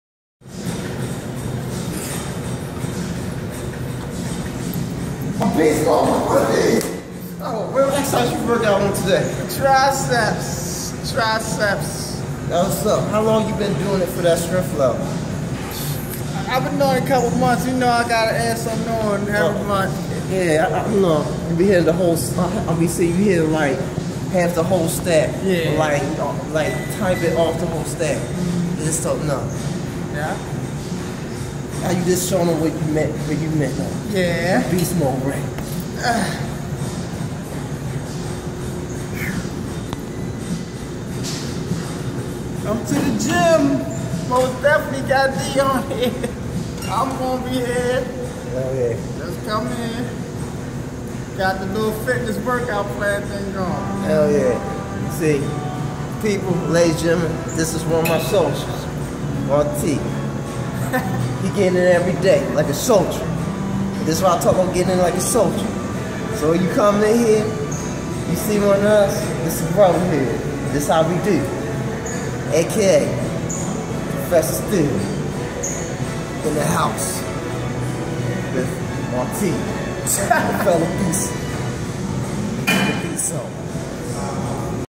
Professor Steel introduces Marquis
Marquis works on his triceps. Professor Steel and Marquis encourages others to go to the gym and Get It In!!!